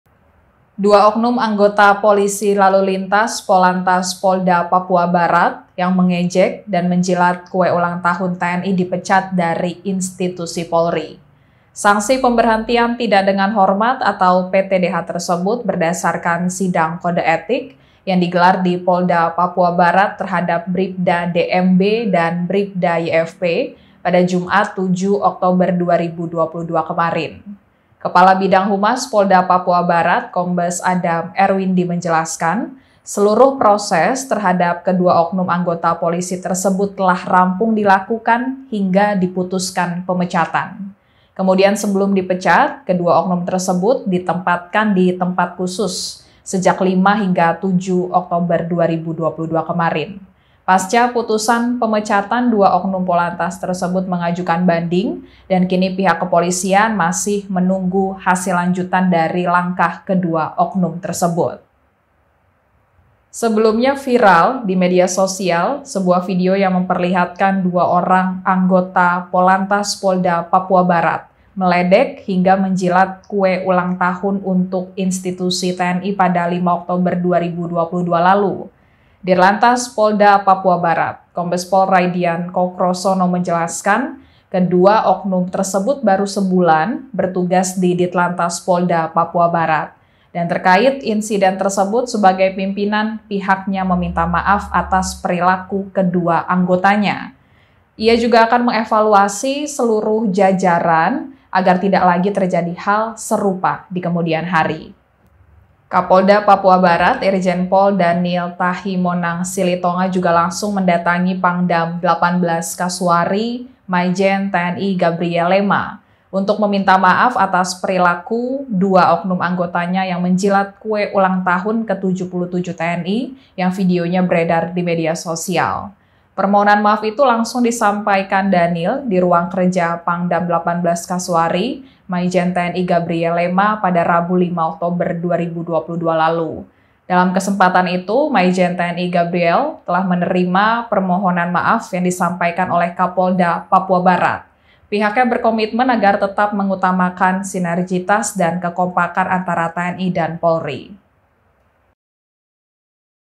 Karier Bripda DMB dan Bripda YFP Tamat di Polisi Karena Jilati Kue HUT ke-77 TNI: Kini Dipecat
TRIBUNNEWS.COM, MANOKWARI -  Bripda DMB dan Bripda YFP, anggota Polantas Polda Papua Barat dipecat buntut viral menjilat kue HUT ke-77 TNI.

Sanksi Pemberhentian Tidak dengan Hormat (PTDH) dijatuhkan setelah sidang etik yang digelar di Polda Papua Barat, pada Jumat (07/10/2022) sekira pukul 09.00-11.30 WIT.

"Kedua terperiksa atas nama Bripda YFP dan Bripda DMB. Hari ini pada pukul 09.00 WIT telah digelar sidang kode etik yang dipimpin Kabid Propam dari hasil sidang telah diputuskan keduanya di-PTDH," kata Kepala Bidang Humas Polda Papua Barat Kombes Adam Erwindi, Jumat.